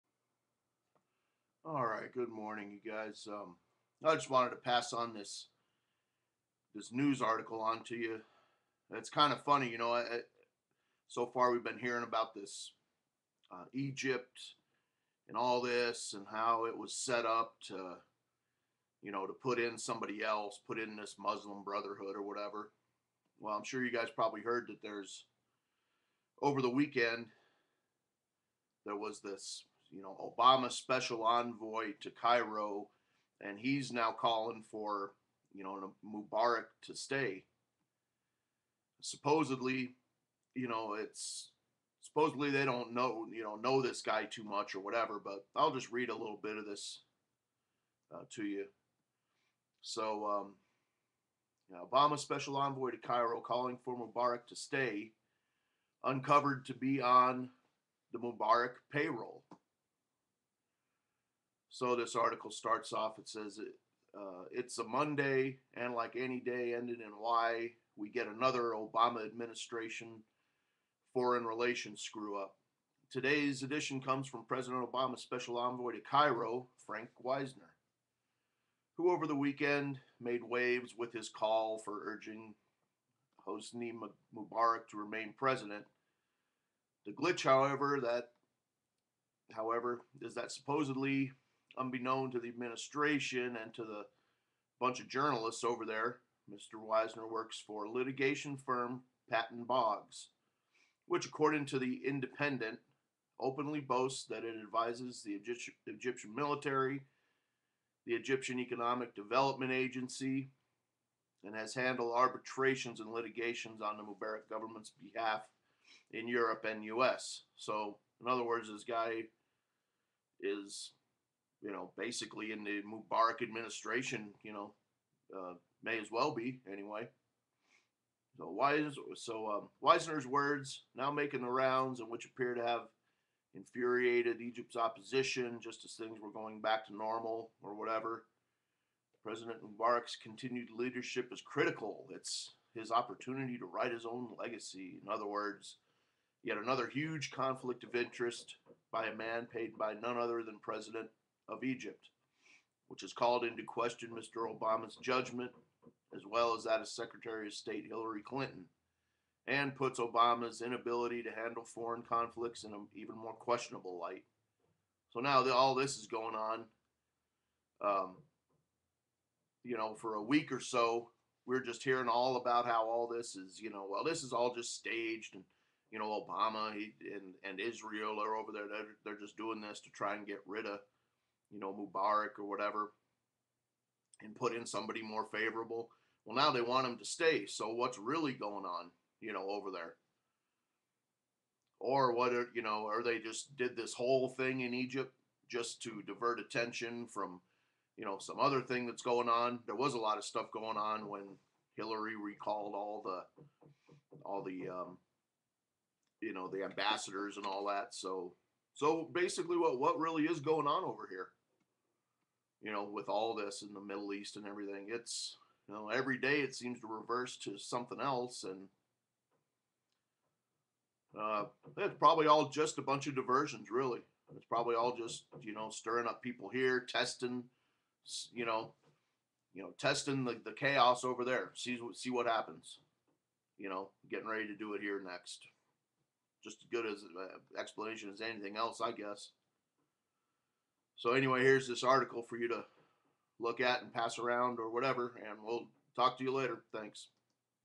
Obamas Mubarack Payroll.
Oh noes! Im sure I'll be arrested by 'feds' for relaying this news and get my channel closed! Oh well, anyway heres the mind twisting news of Obama and Hillary total gross incompetence on display, or is it they just dont care at all how open their corruption and fraud is due to a media which will never point any of it out?